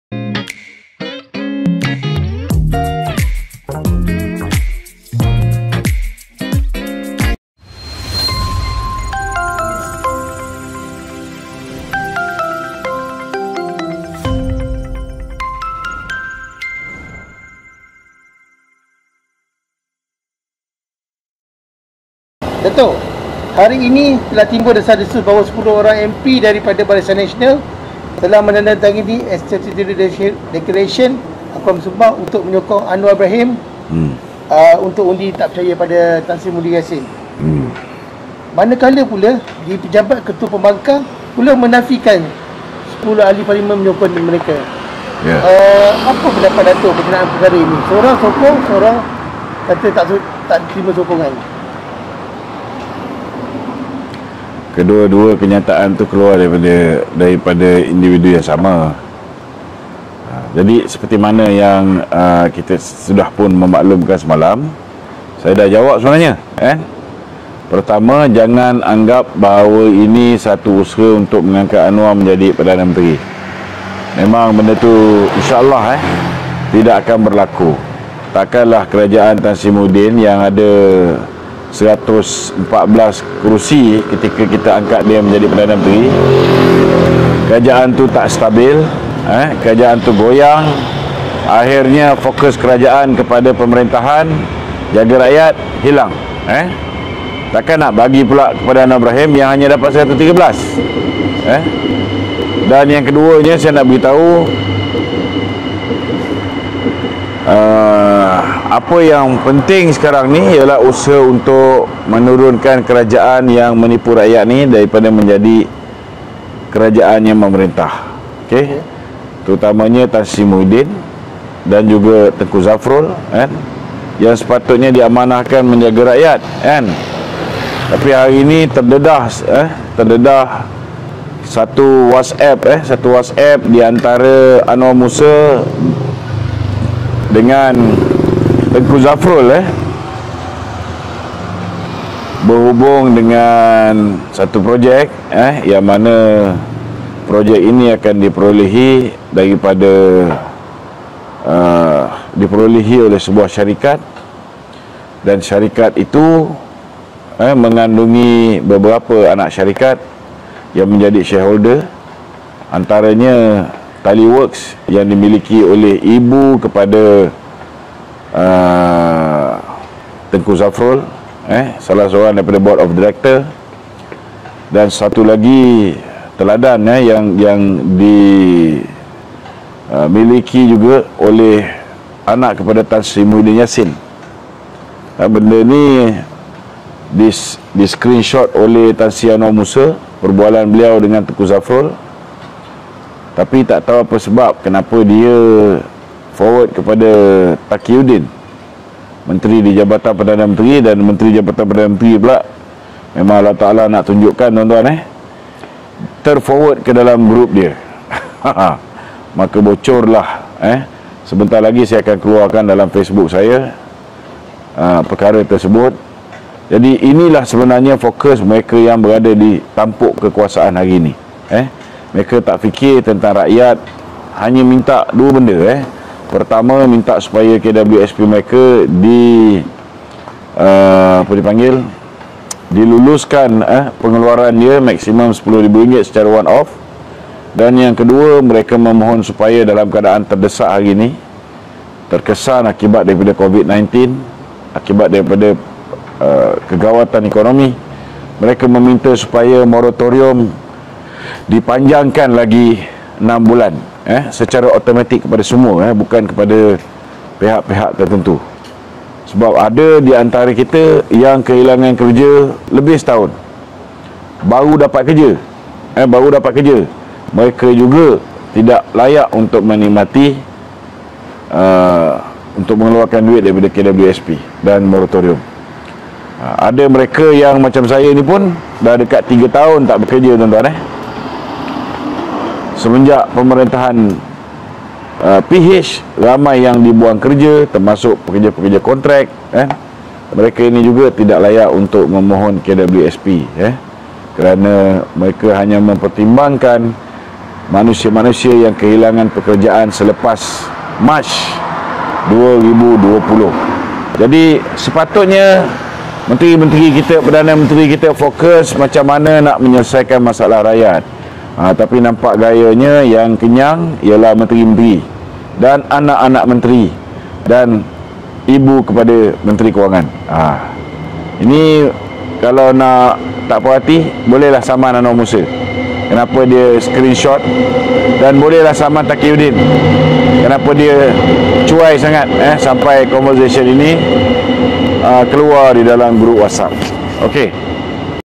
Kenyataan MP UMNO Bercanggah Soal Sokongan Pada Anwar. Zafrul Korup ?
Terdapat percanggahan mengenai sokongan MP UMNO terhadap sokongan kepada Anwar Ibrahim. Apa yang benar sebenarnya?
Tengku Zafrul disabitkan dengan satu screenshot whatsapp yang didapati berbual tentang pemberian projek kepada keluarga terdekat.